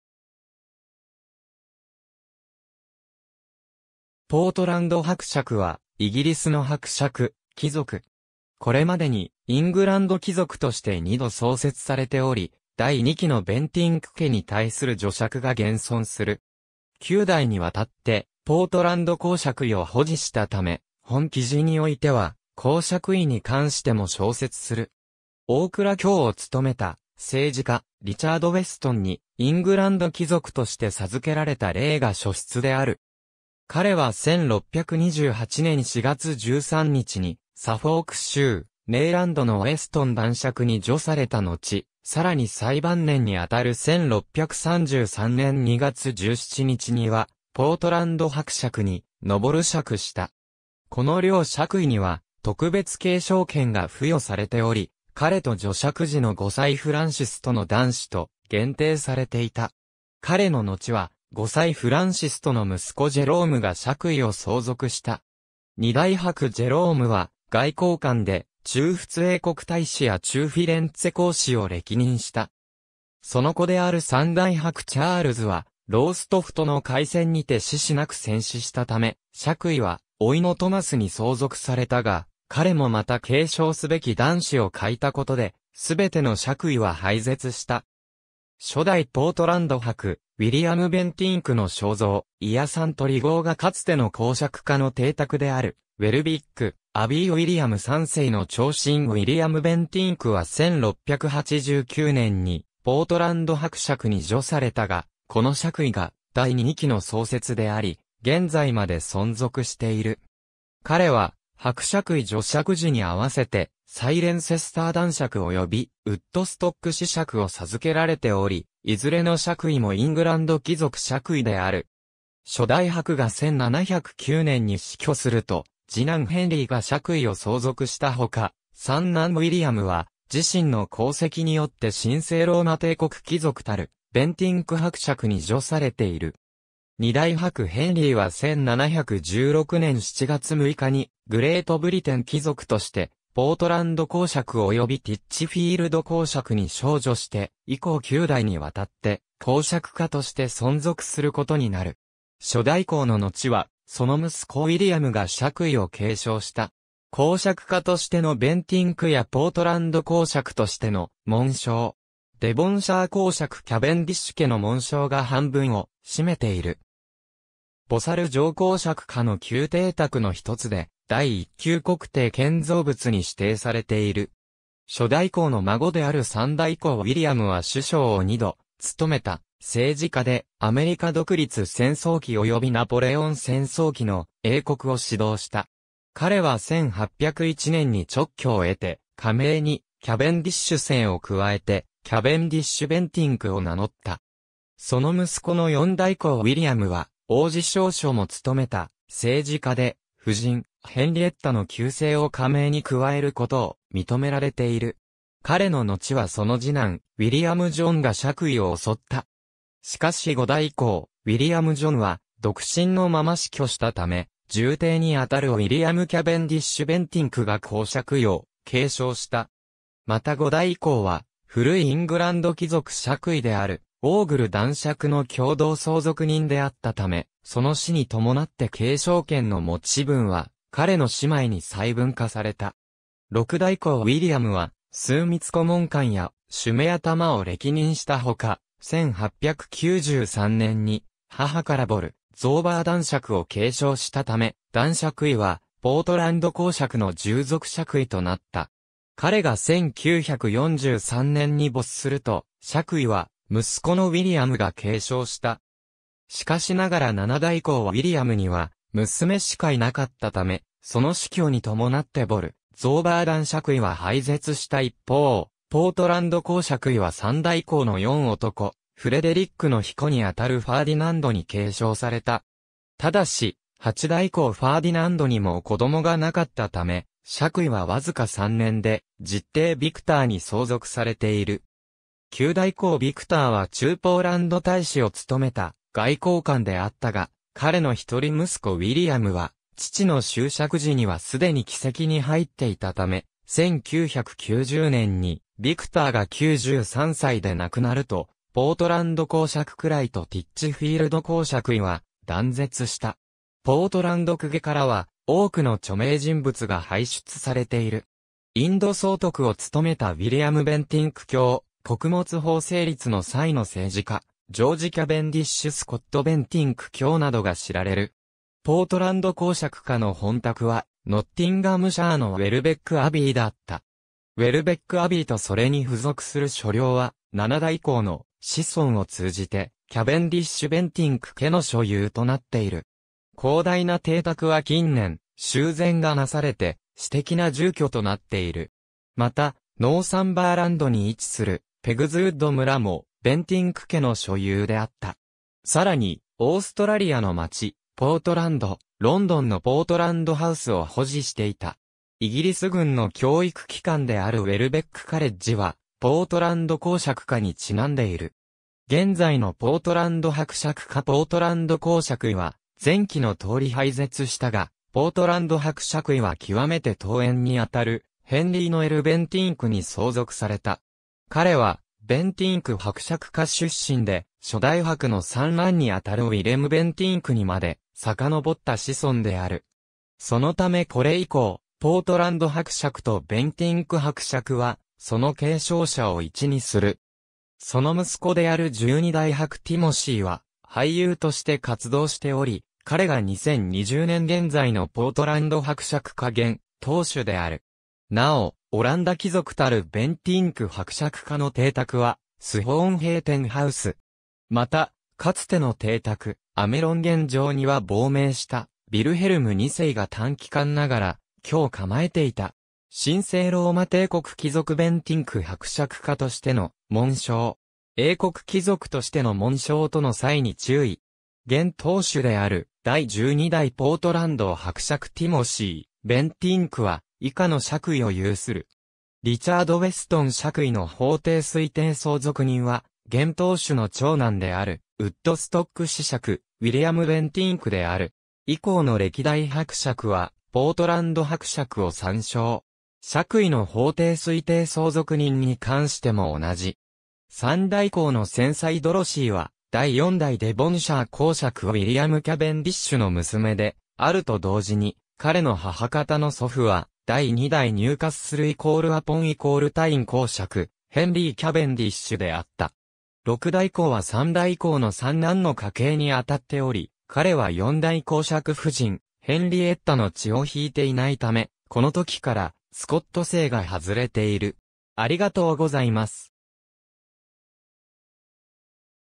ポートランド伯爵
ポートランド伯爵（英: Earl of Portland）はイギリスの伯爵、貴族。
これまでにイングランド貴族として2度創設されており、第2期のベンティンク家に対する叙爵が現存する。
9代にわたってポートランド公爵位を保持したため、本記事においては公爵位に関しても詳説する。
大蔵卿を務めた政治家リチャード・ウェストン(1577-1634?)にイングランド貴族として授けられた例が初出である。
彼は1628年4月13日にサフォーク州ネイランドのウェストン男爵(Baron Weston, of Neyland in the County of Suffolk)に叙されたのち、さらに最晩年にあたる1633年2月17日にはポートランド伯爵(Earl of Portland)に陛爵した。
この両爵位には特別継承権が付与されており、彼と叙爵時の後妻フランシス(?-1645)との男子と限定されていた。
彼ののちは、後妻フランシスとの息子ジェロームが爵位を相続した。
2代伯ジェローム(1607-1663)は外交官で駐仏英国大使や駐フィレンツェ公使を歴任した。
その子である3代伯チャールズ(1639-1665)はローストフトの海戦にて嗣子なく戦死したため、爵位は甥のトマス(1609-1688)に相続されたが、彼もまた継承すべき男子を欠いたことですべての爵位は廃絶した。
初代ポートランド伯ウィリアム・ベンティンクの肖像、イアサント・リゴー画 かつての公爵家の邸宅であるウェルビック・アビー ウィリアム3世の寵臣ウィリアム・ベンティンク(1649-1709)は1689年にポートランド伯爵(Earl of Portland)に叙されたが、この爵位が第2期の創設であり現在まで存続している。
彼は伯爵位叙爵時に併せてサイレンセスター男爵（Baron Cirencester）及びウッドストック子爵（Viscount Woodstock）を授けられており、いずれの爵位もイングランド貴族爵位である。
初代伯が1709年に死去すると次男ヘンリーが爵位を相続したほか、三男ウィリアム(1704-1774)は自身の功績によって神聖ローマ帝国貴族たるベンティンク伯爵(Count Bentinck)に叙されている。
2代伯ヘンリー(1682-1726)は1716年7月6日にグレートブリテン貴族としてポートランド公爵（Duke of Portland）及びティッチフィールド侯爵（Marquess of Titchfield）に昇叙して、以降9代にわたって公爵家として存続することになる。
初代公ののちは、その息子ウィリアムが爵位を継承した。
公爵家としてのベンティンク家 ポートランド公爵としての紋章。
デヴォンシャー公爵キャヴェンディッシュ家の紋章が半分を占めている。
ボサル城 公爵家の旧邸宅の一つで、第一級国定建造物に指定されている。
初代公の孫である3代公ウィリアム(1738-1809)は首相を2度務めた政治家で、アメリカ独立戦争期及びナポレオン戦争期の英国を指導した。
彼は1801年に勅許を得て、家名にキャヴェンディッシュ姓を加えてキャヴェンディッシュ＝ベンティンク（Cavendish-Bentinck）を名乗った。
その息子の4代公ウィリアム(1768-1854)は王璽尚書も務めた政治家で、夫人ヘンリエッタの旧姓(スコット姓)を家名に加えることを認められている。
彼ののちはその次男ウィリアム・ジョンが爵位を襲った。
しかし5代公ウィリアム・ジョン(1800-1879)は独身のまま死去したため、従弟にあたるウィリアム・キャヴェンディッシュ＝ベンティンクが公爵位を継承した。
また5代公は古いイングランド貴族爵位であるオーグル男爵の共同相続人であったため、その死に伴って継承権の持分は彼の姉妹に細分化された。
6代公ウィリアム(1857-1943)は枢密顧問官や主馬頭を歴任したほか、1893年に母からボルゾーバー男爵を継承したため、男爵位はポートランド公爵の従属爵位となった。
彼が1943年に没すると、爵位は息子のウィリアムが継承した 。
しかしながら7代公ウィリアム(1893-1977)には娘しかいなかったため、その死去に伴ってボルゾーバー男爵位は廃絶した一方、ポートランド公爵位は3代公の四男フレデリックの曾孫にあたるファーディナンドに継承された。
ただし、8代公ファーディナンド(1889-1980)にも子供がなかったため、爵...